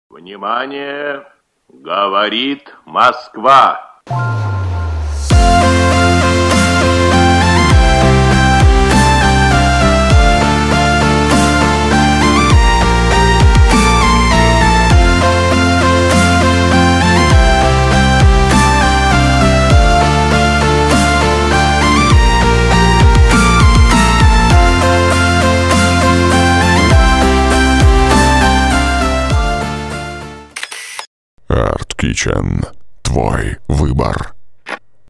DJI video
Прекрасные летние виды Москвы.
Съемки с воздуха на DJI Mavic Pro.